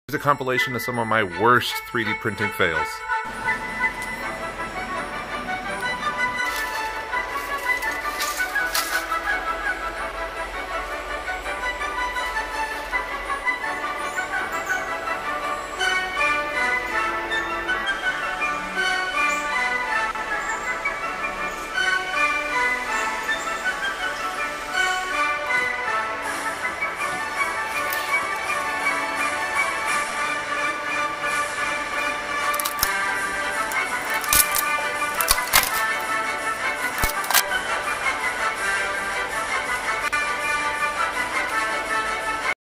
Some of my WORST 3d printing fails
Today I'll show you some of my worst 3d printing fails. Most of these are caused by a part breaking or malfunctioning.

My recommended 3D Printer